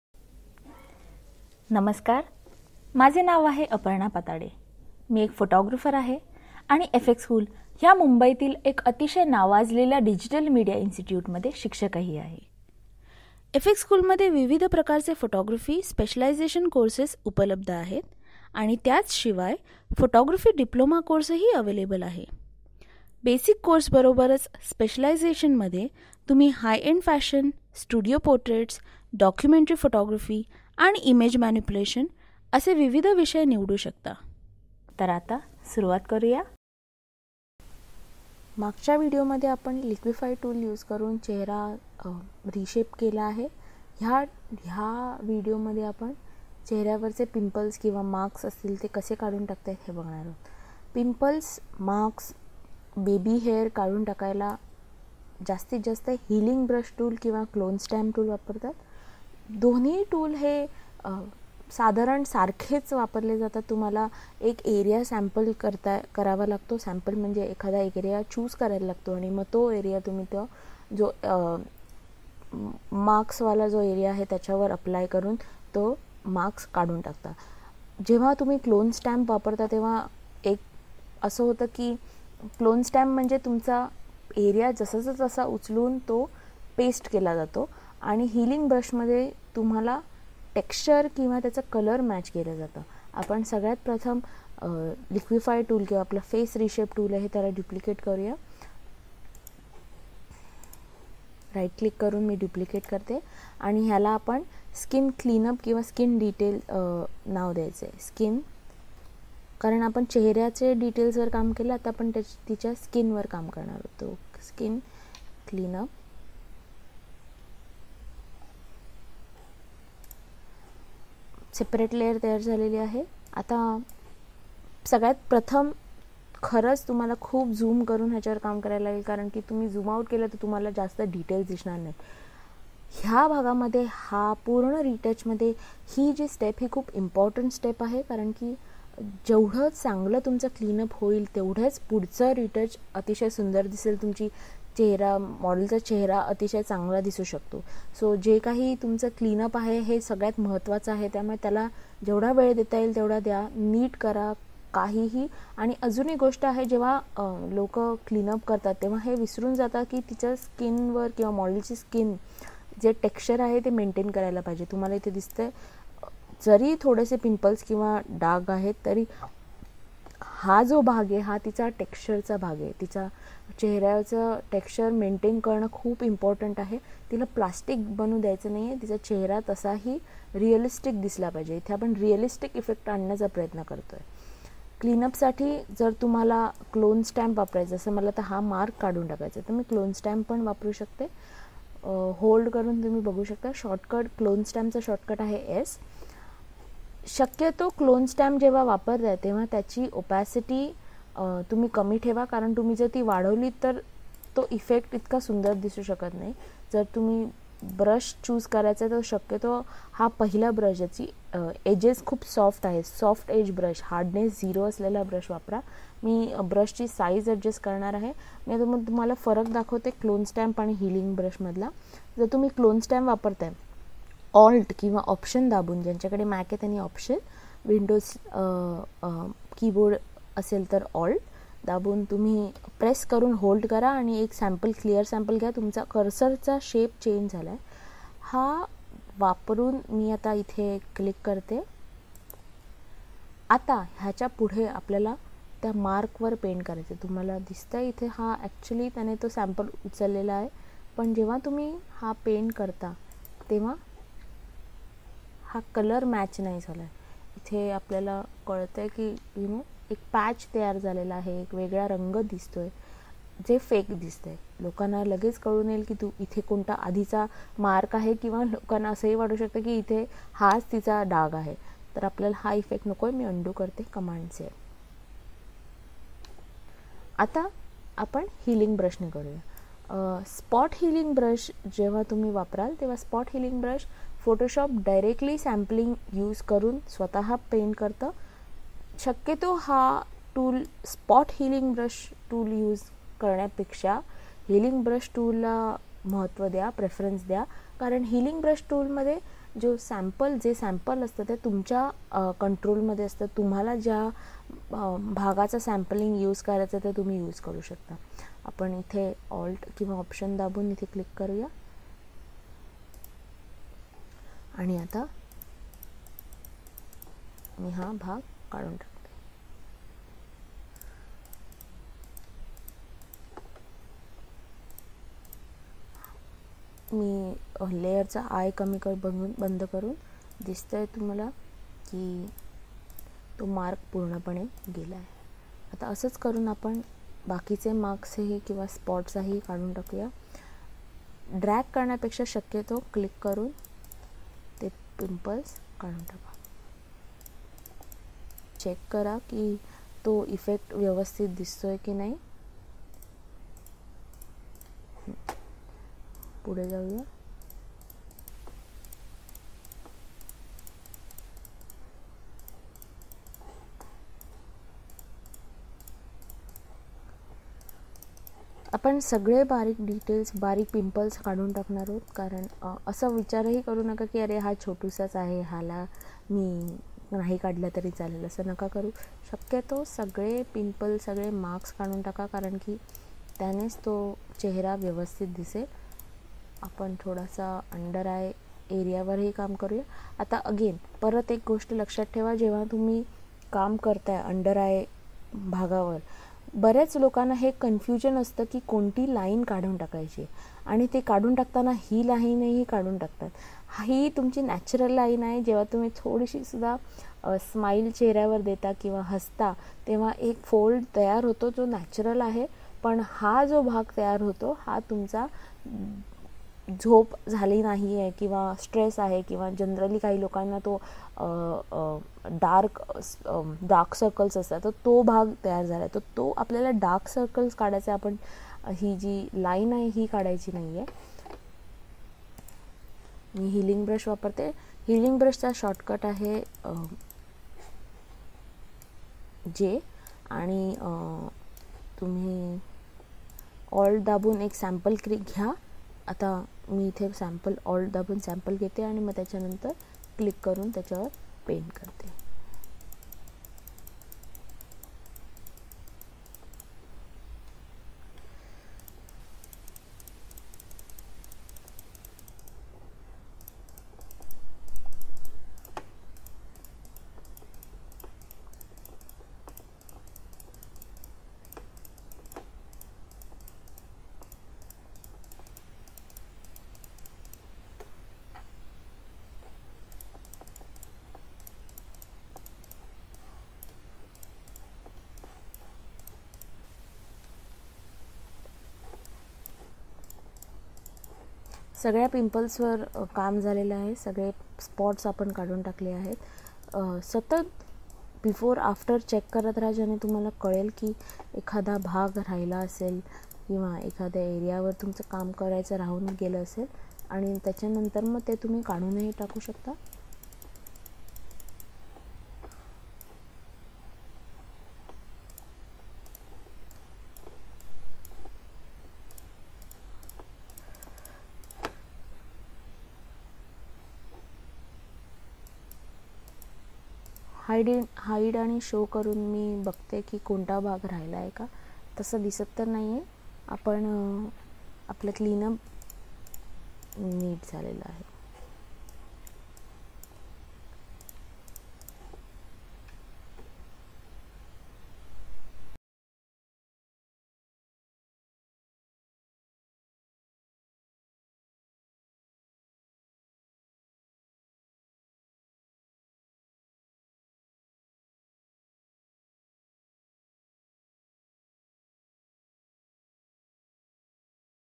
Free Retouch Tutorial - Cleaning up Pimples and Blemishes (Video 6)
FX School Photography presents a free Marathi tutorial series on high quality photo retouching using Adobe Photoshop.

This video teaches you how to clean up pimples and blemishes.